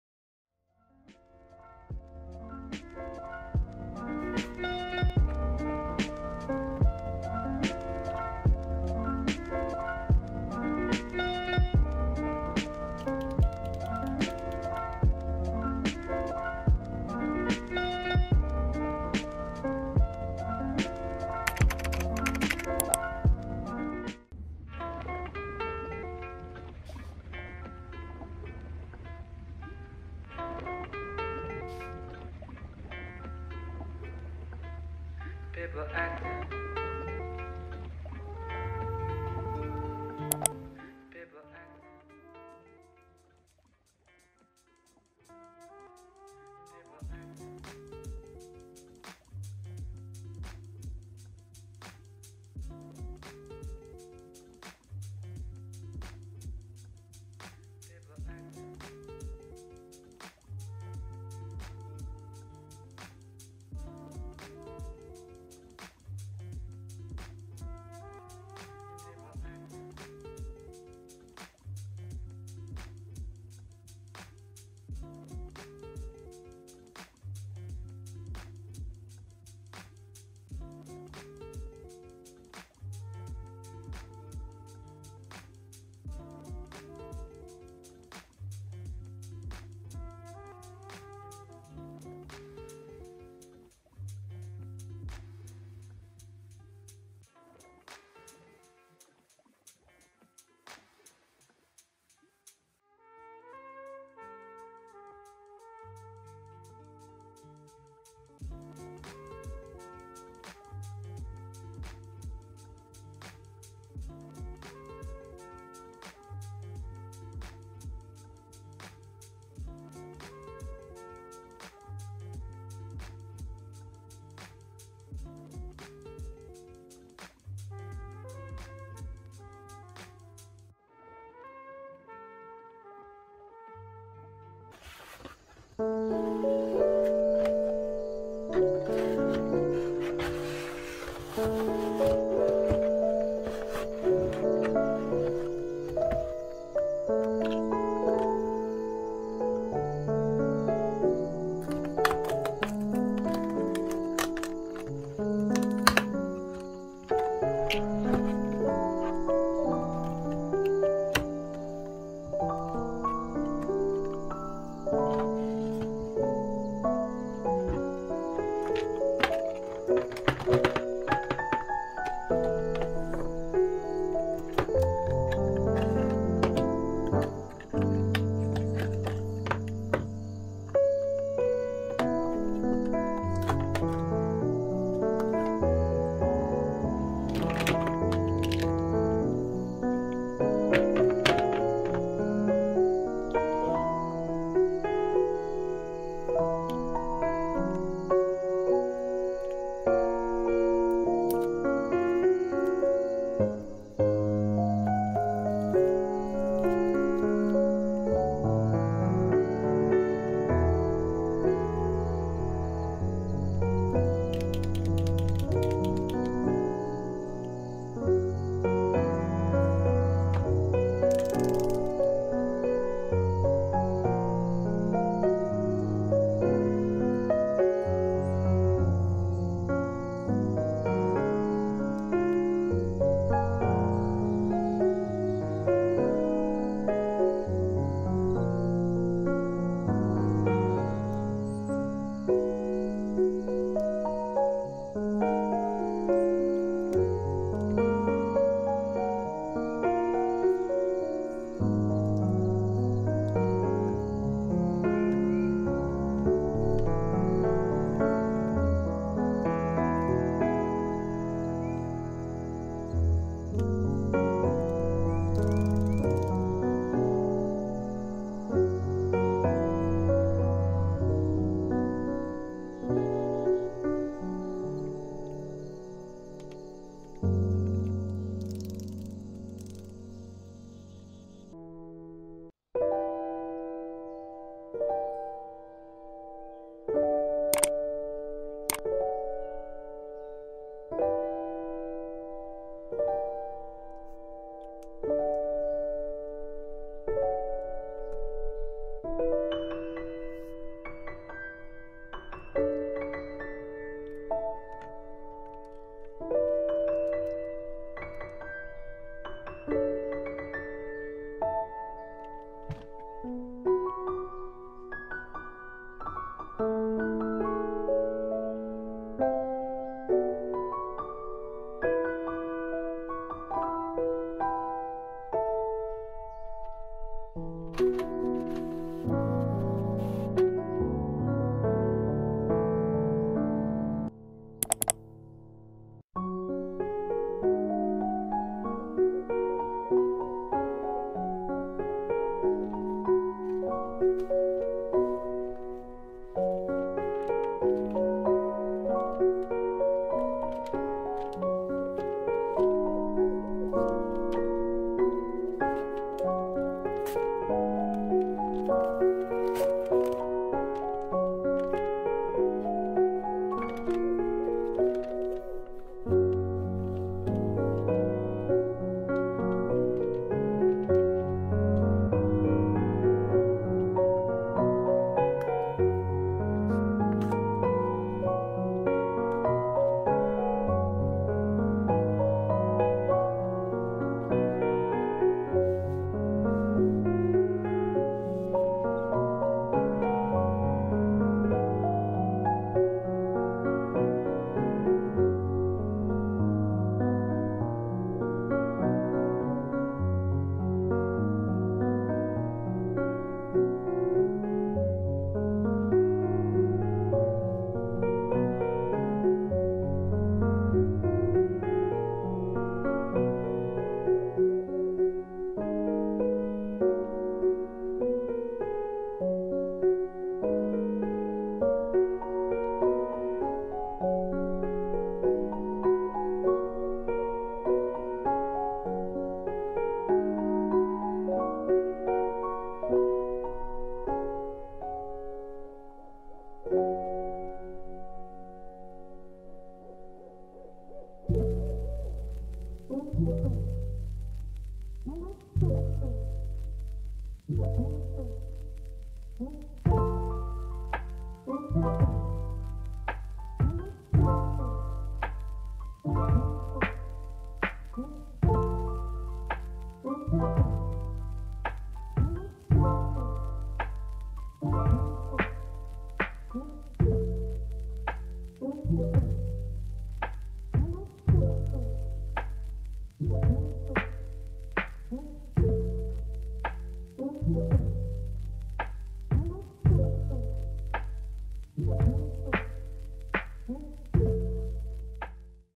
Studio Vlog 002 🌱 Small Business Haul, Supplies + Making Custom Envelopes✉
Click Me!
Hellooo and welcome back to my channel :D If you are new here my name is Emma and I run a small sticker business that I plan to expand into stationary supplies :3 I hope you enjoy my vlog and if you did let me know through liking/commenting/subscribing - it means a lot :) x

♡ LINKS TO ITEMS/PEOPLE MENTIONED IN THE VIDEO:
Holographic vinyl